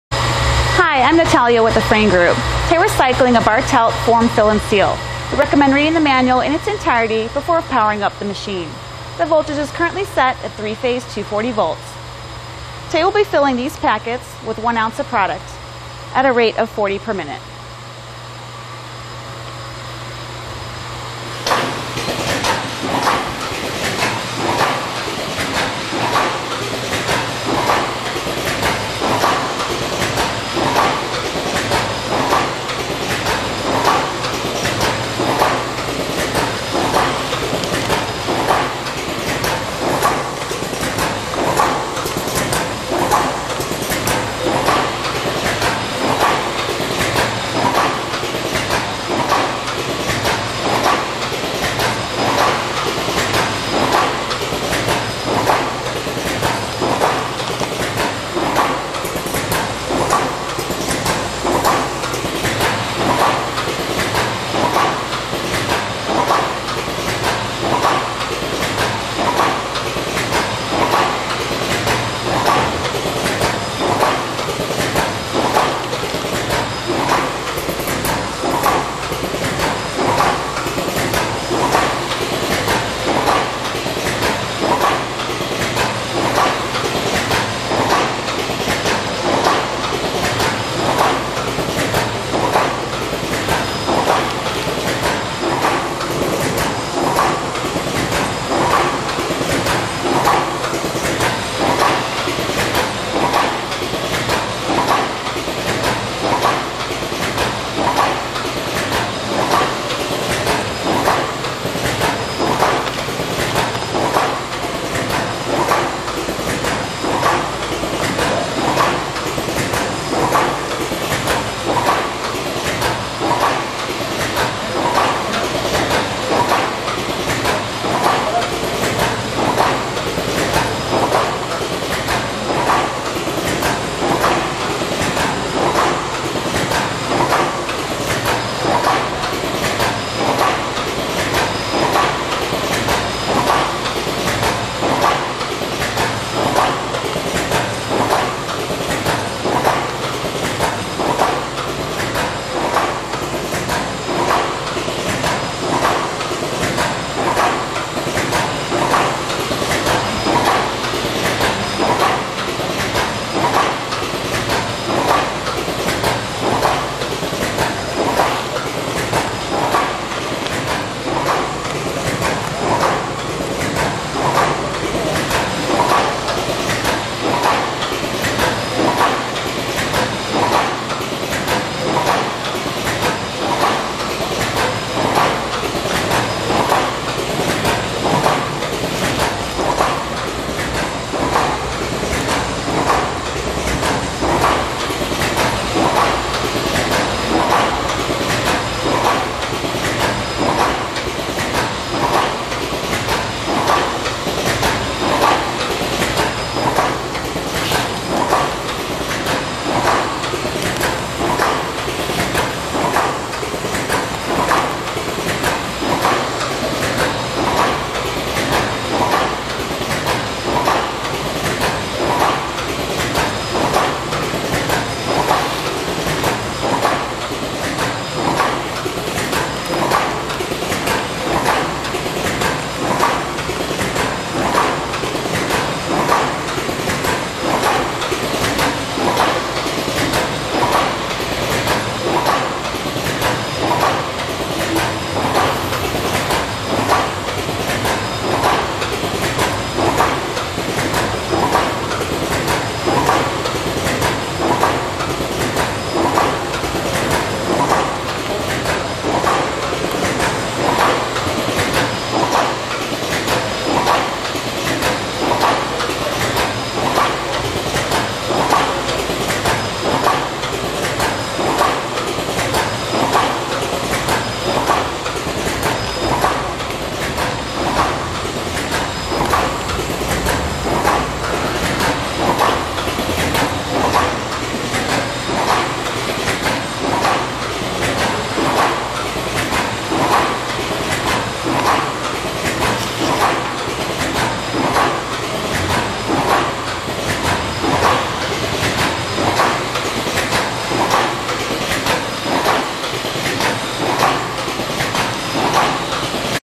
Bartelt, horizontal, intermittent motion, dual auger,form/fill and seal
Bartelt Model IM714, automatic, horizontal, intermittent motion, dual auger, mechanical driven, form, fill, seal machine rated from 20 to 100 pouches per minute - depending on materials and application. Pouch size range: 2" to 5-3/4" in Width; 2" to 9 -1/2" in Length. Equipped with chain feed conveyor with 14 stations set on 7" centers, forming plow, film tension rollers, side seal and cut-off station, suction cup style pouch opener, (2)  auger fillers with (1) heat seal bar with pouch pick-off station and 18" OD maximum film roll, with 20" maximum web width, on 1" OD core. Control panel has A/B PLC controller and swing arm touch screen controls with readout, vari speed, has top, bottom and side heat seal controls, start, stop, web feed and e-stop push buttons, safety guarding and mounted on heavy duty base frame.